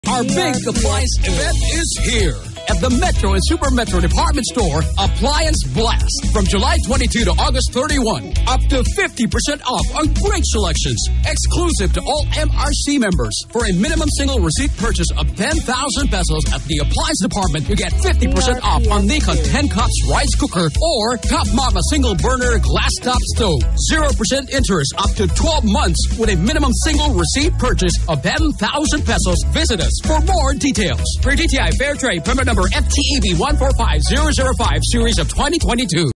Metro Department Store Appliance Blast Promo Radio Ad August 2022 36s
This is the August 2022 Radio Commercial for Metro Department Store's Appliance Blast Promo.

DRPS Two 2270: Metro Department Store Appliance Blast Promo Radio Ad August 2022 36s